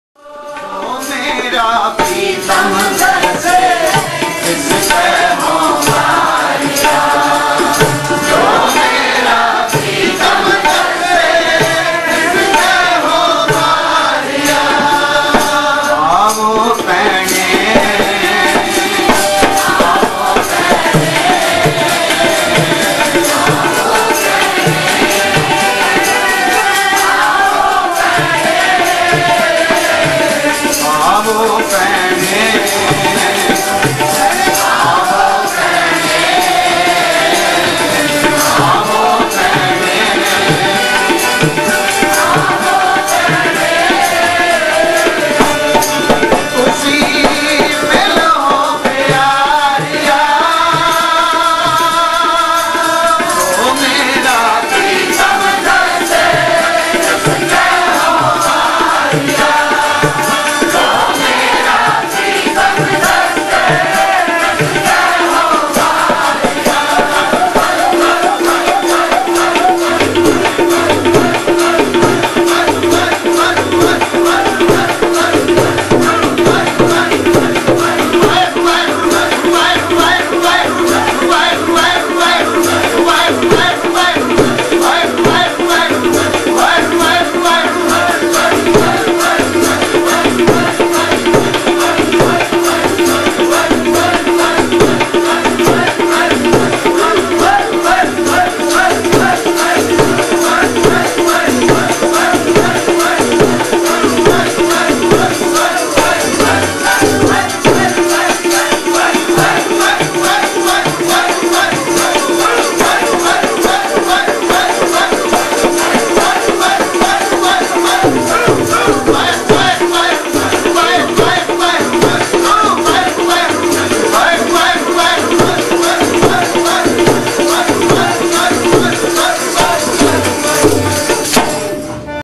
waheguru simran bhai jagpal singh ji
This is in Toronto in July 2008
Visit shabadkirtan.yolasite.com for gurbani and gurbani.